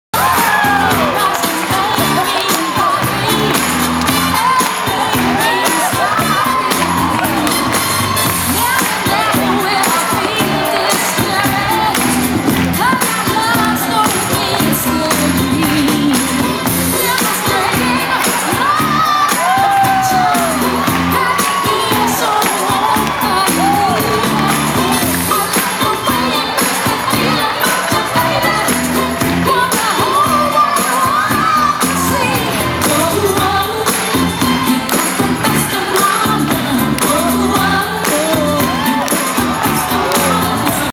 Youth Specialties Convention 2008 in Sacramento - Yaconelli
Mike Yaconelli dancing on stage after giving his message at YS.